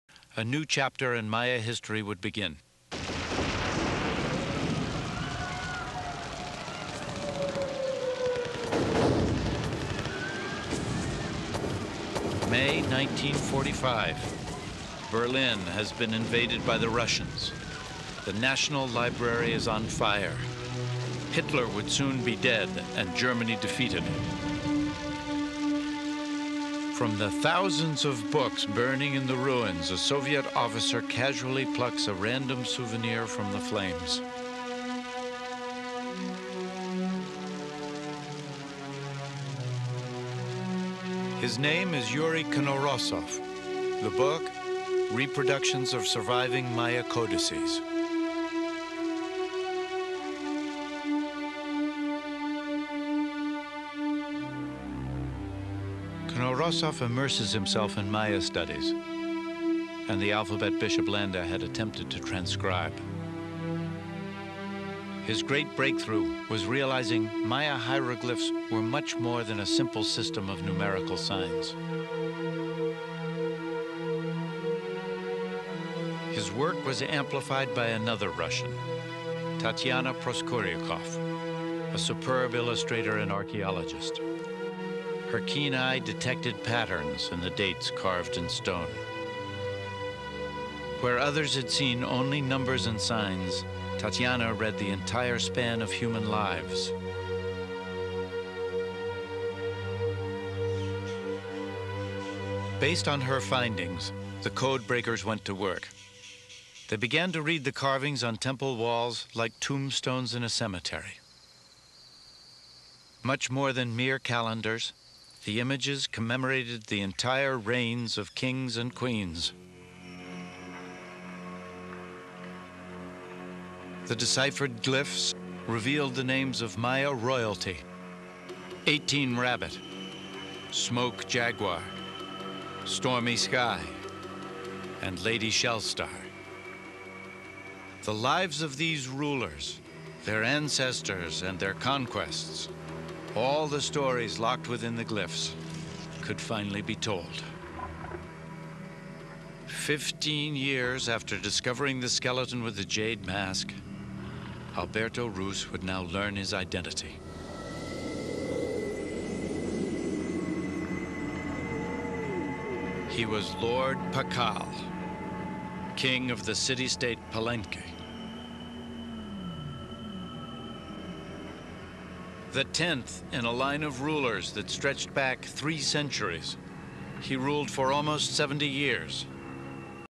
Breaking the code of the Maya Language - Yuri Knorozov, Tatiana Proskouriakoff
The Russians helped to be able to read the Maya scripts